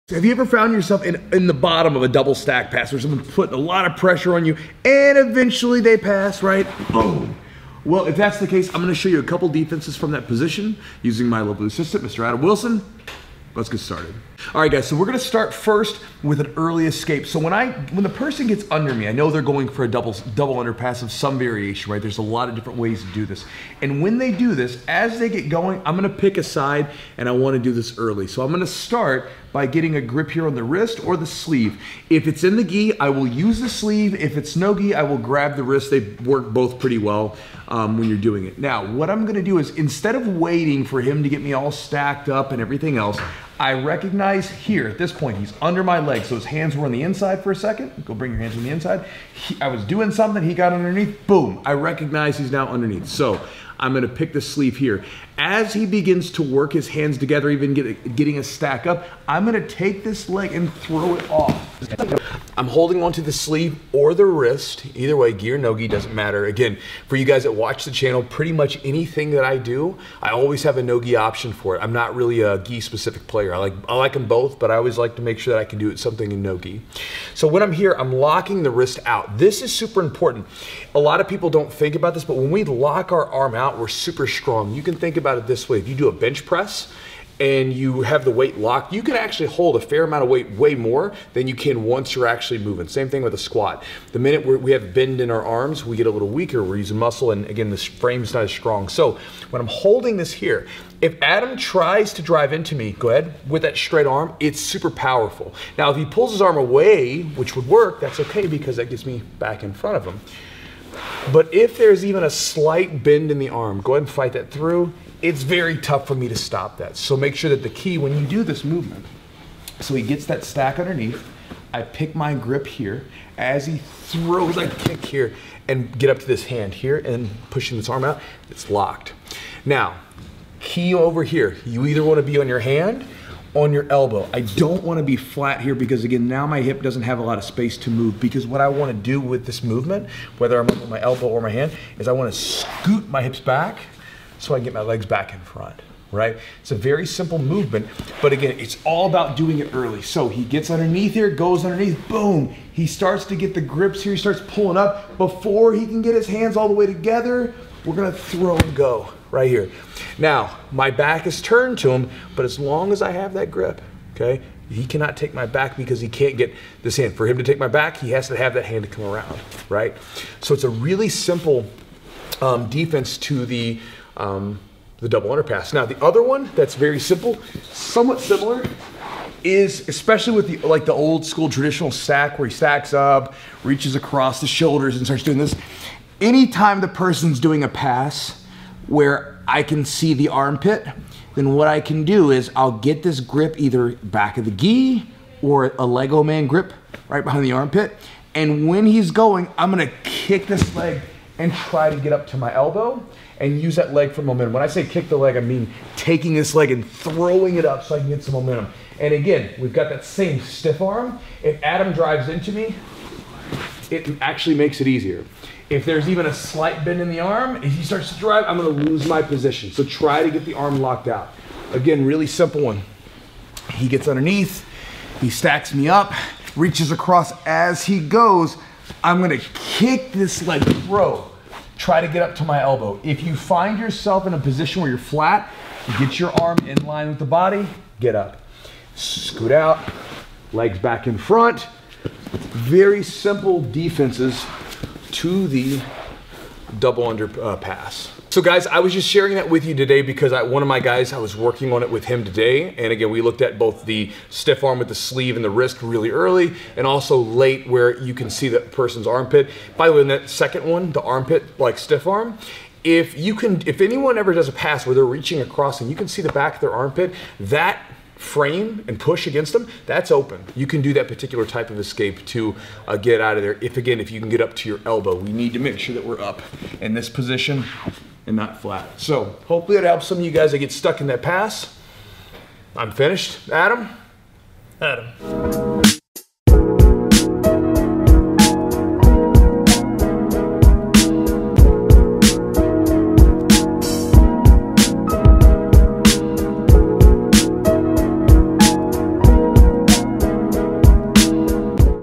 Basic Double Stack Pass Counter in BJJ
In today's video I'll be reviewing a couple options to defend the double stack pass.

One of my guys was having trouble defending it today in class, so I thought I'd share what we discussed here too.

This is a pretty common pass in Brazilian Jiu-jitsu, both Gi and No-Gi. In fact, it's also one of my personal favorite passes.

With that being said, every BJJ technique has counters and defenses & I wanted to show you a few of my favorite options here.

Getting stacked sucks. So, hopefully there's a few ideas here that you find helpful.